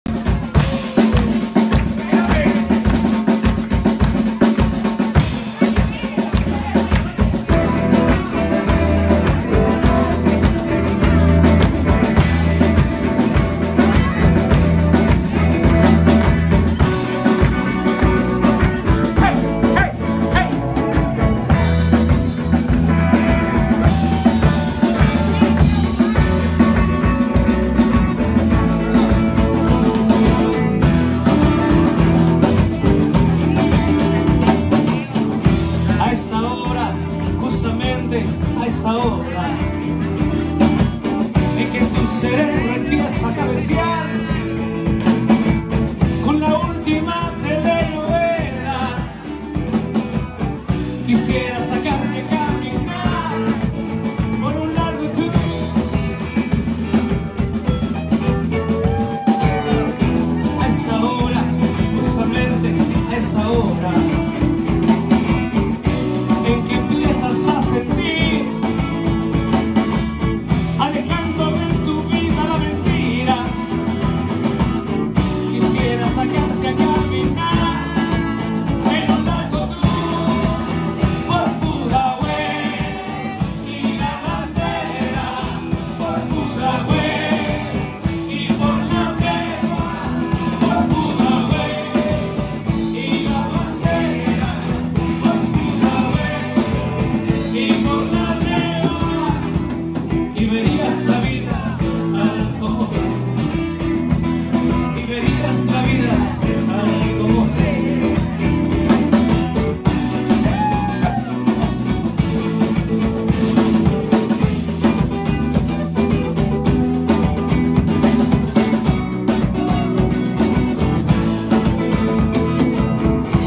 Chile Lindo 25th Anniversary Party - video #1
Axel Herrera, drums & vocals, Mauricio Bertín, guitar & vocals, Adrián Dobarro,, bass & vocals.